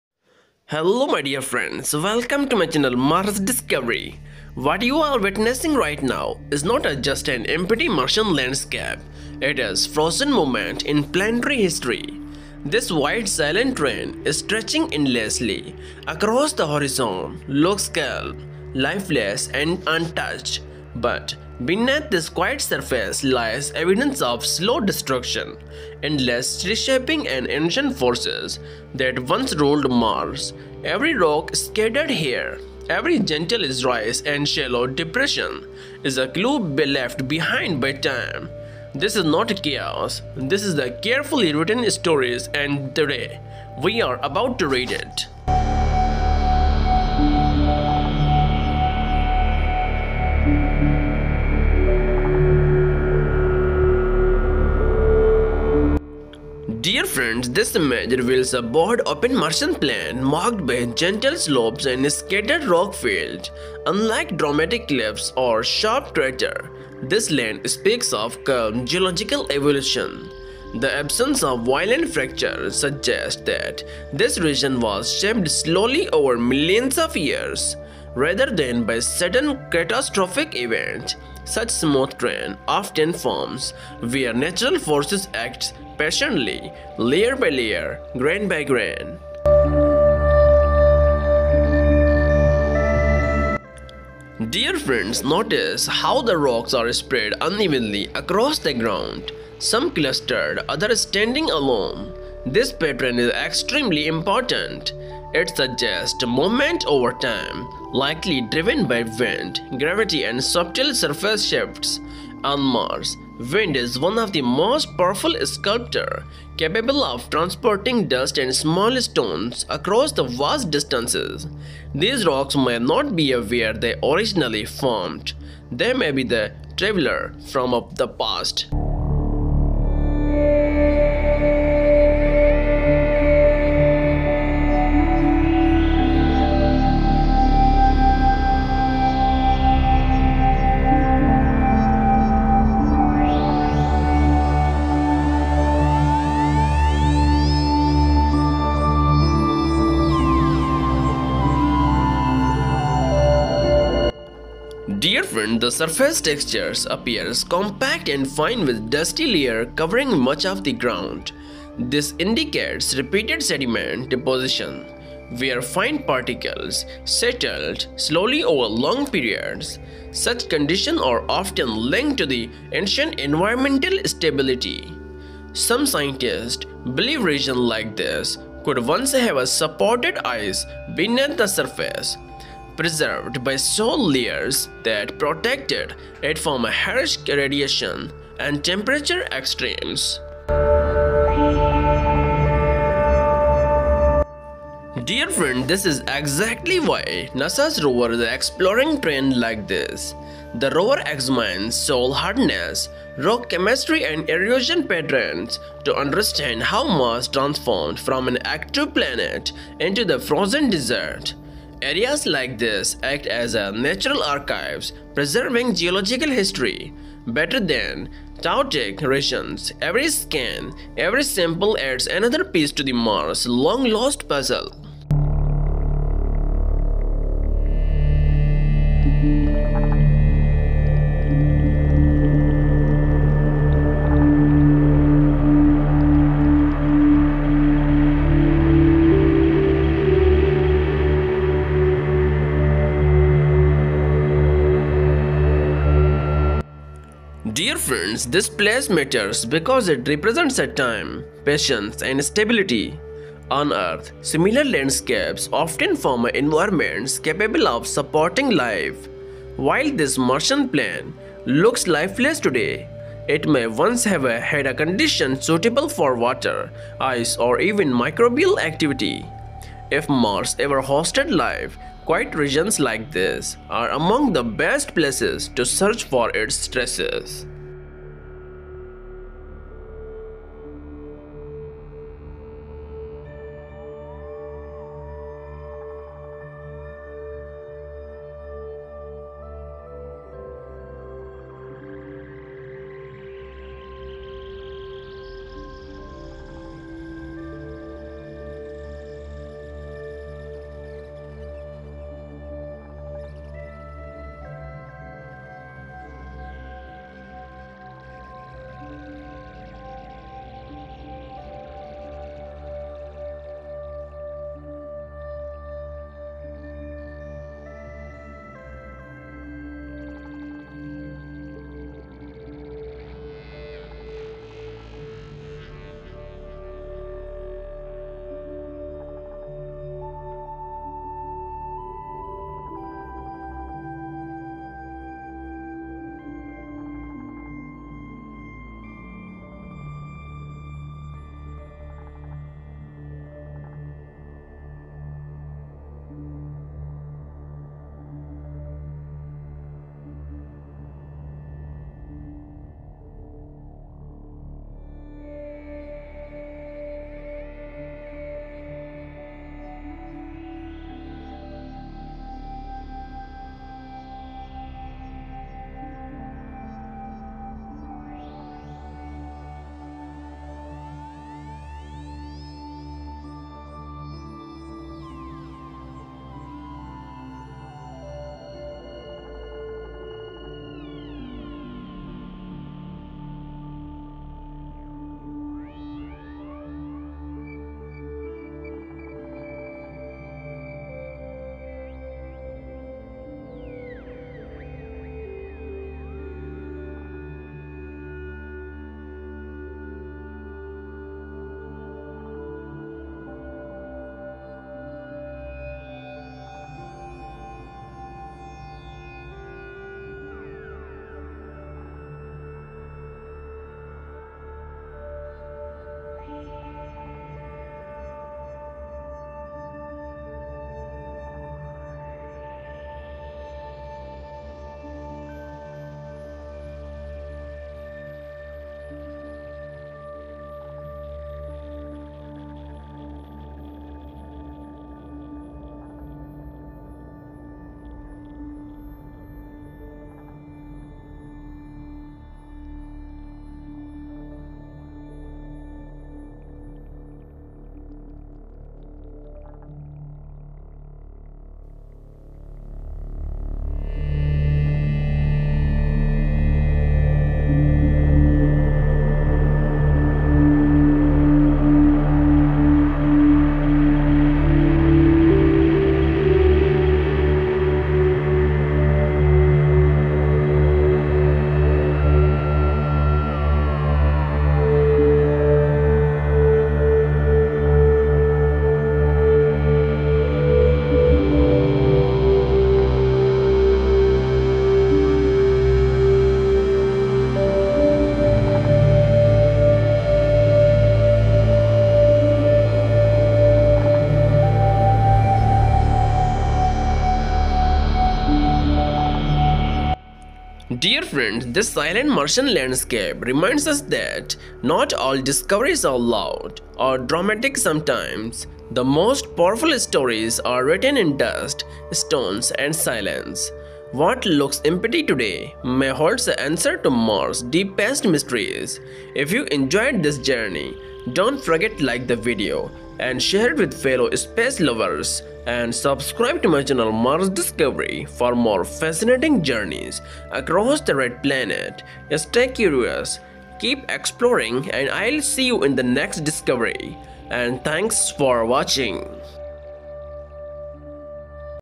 NASA’s Rover Captures Stunning & Mysterious Mars Landscape | Amazing 4K Footage of red planet Mars
Join us as we explore the mysteries of Mars through stunning footage captured by NASA’s Mars Rovers. Every discovery takes us one step closer to uncovering the secrets of the Red Planet!"